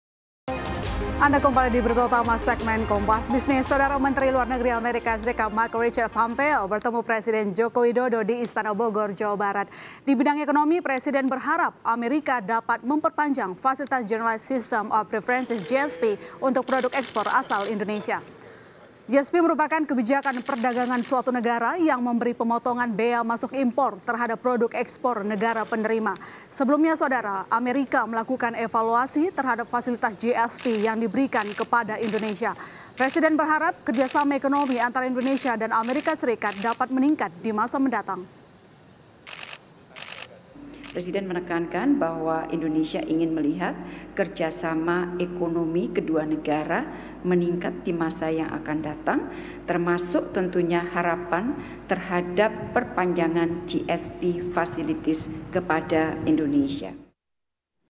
Terkait Bidang Ekonomi, Ini Pembahasan Jokowi dan Menlu AS Mike Pompeo
KOMPAS.TV - Menteri Luar Negeri Amerika Serikat, Michael Richard Pompeo bertemu presiden Joko Widodo di Istana Bogor, Jawa Bara (29/10/2020).

Dalam pertemuan tersebut,  Jokowi menyebut kunjungan Mike Pompeo di tengah pandemi Covid-19 tersebut memiliki makna yang mendalam dalam hubungan bilateral Indonesia dan Amerika Serikat.

Tak hanya itu, kegiatan saling kunjung antar pejabat Amerika dan Indonesia juga disebut Jokowi paling intensif dilakukan, dibandingkan dengan negara-negara lain.

"Kunjungan Anda di tengah pandemi ini menunjukkan arti penting kemitraan strategis antara Indonesia dan Amerika. Selama ini, saling kunjung antar pejabat kita cukup intensif bahkan dapat saya sampaikan paling intensif" ungkap Jokowi kepada Mike Pompeo saat bertemu  di Istana Bogor (29/10).

Sementara itu, Amerika menyebut peran penting Indonesia di dalam kawasan Asia Tenggara.

Sebagai negara dengan penduduk muslim terbesar di dunia, dengan luas terbesar di Asia Tenggara, Indonesia dinilai memerankan peranan penting di kawasan.

Di bidang ekonomi, Presiden berharap Amerika dapat memperpanjang  fasilitas Generalized System of Preferences (GSP) untuk produk ekspor asal Indonesia.

GSP merupakan kebijakan perdagangan suatu negara yang memberi pemotongan bea masuk impor terhadap produk ekspor negara penerima.

Sebelumnya, Amerika melakukan evaluasi terhadap fasilitas GSP yang diberikan kepada Indonesia.

Presiden berharap, kerja sama ekonomi antara Indonesia dan Amerika Serikat dapat meningkat di masa mendatang.